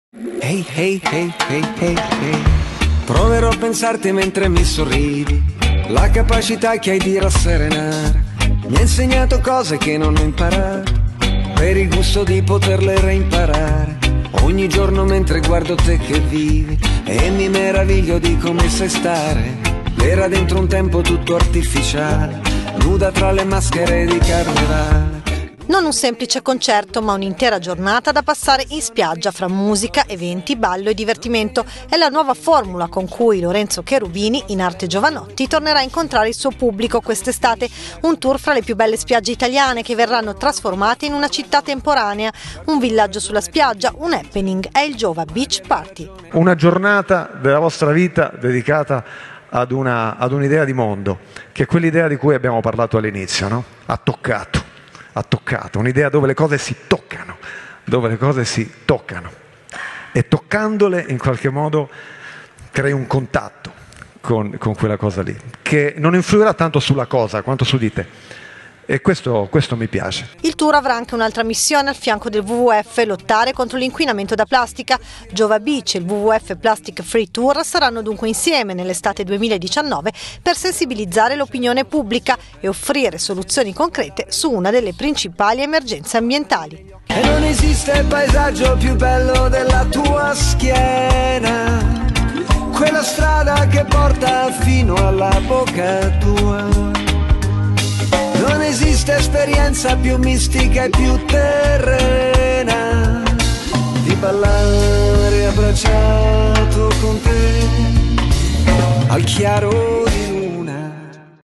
Jovanotti trasforma l'estate in una grande festa sulla spiaggia
Milano, (askanews) - Non un semplice concerto, ma un'intera giornata da passare in spiaggia fra musica, eventi, ballo e divertimento. È la nuova formula con cui Lorenzo Jovanotti tornerà a incontrare il suo pubblico quest'estate. Un tour fra le più belle spiagge italiane che verranno trasformate in una città temporanea, un villaggio sulla spiaggia, un happening. È il "Jova beach party".
"Una giornata della vostra vita dedicata a una idea di mondo, una giornata dove le cose si toccano e toccandole in qualche modo crei un contatto con quella cosa lì, che non influirà
tanto sulla cosa quanto su di te e questo mi piace".
Il tour avrà anche un'altra missione al fianco del Wwf: lottare contro l'inquinamento da plastica.
JOVA BEACH e il WWF PlasticFree Tour, saranno quindi insieme nell'estate 2019 per sensibilizzare l'opinione pubblica e offrire soluzioni concrete su una delle principali emergenze ambientali.